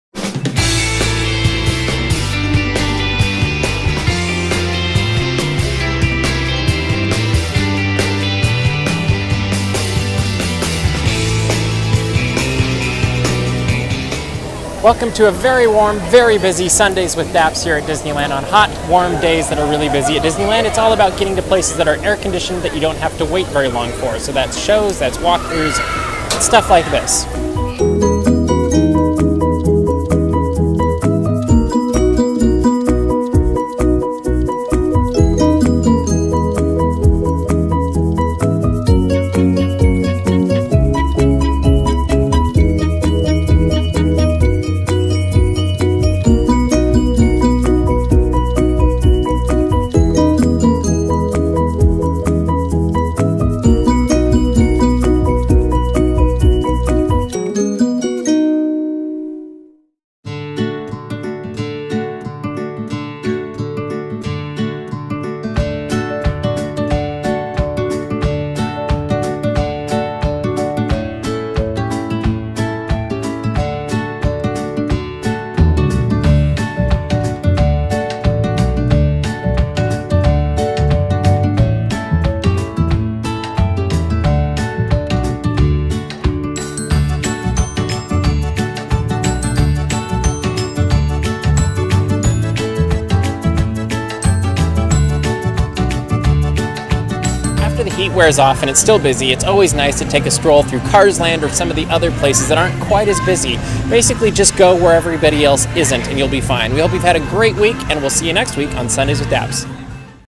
Staying Cool at Disneyland on a Busy Day - Sundays with DAPs
On this Sundays with DAPs, Mr. DAPs shares how to survive a hot day at Disneyland and also have some fun when it is both hot and busy at the Disneyland Resort! d